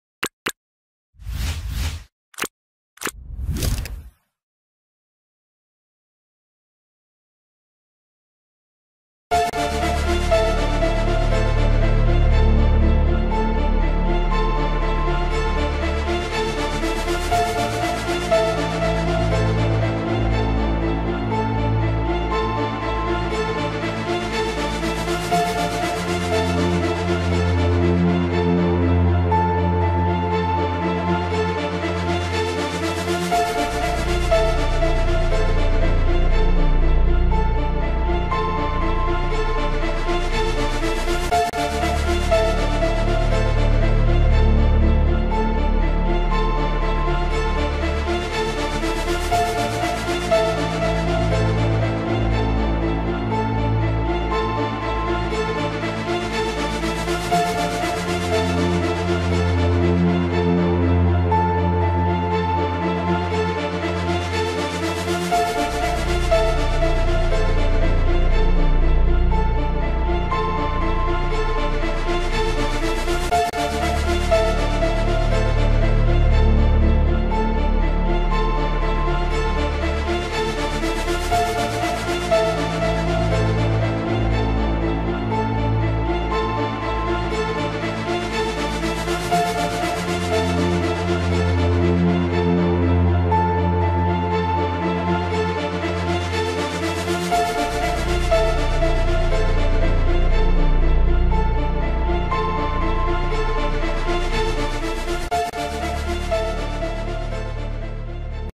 Top 10 Quotes By Adele
Adele Laurie Blue Adkins MBE is an English singer and songwriter. After graduating from the BRIT School for Performing Arts and Technology in 2006, Adele was given a recording contract by XL Recordings